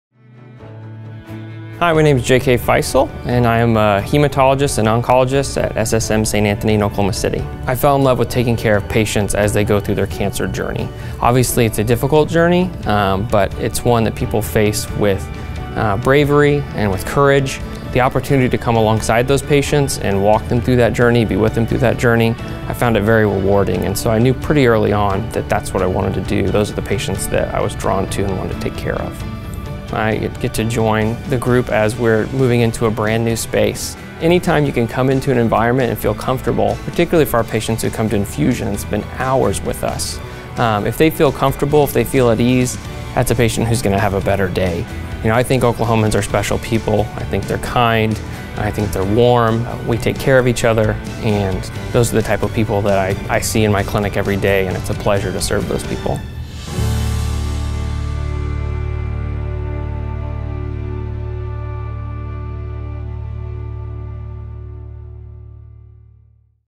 Meet J.K. Feisal, MD
SSM Health Medical Group is excited to welcome hematologist/oncologist James K. “J.K.” Feisal, MD. Dr. Feisal is passionate about providing exceptional care to everyone in our community affected by cancer. He is now accepting patients.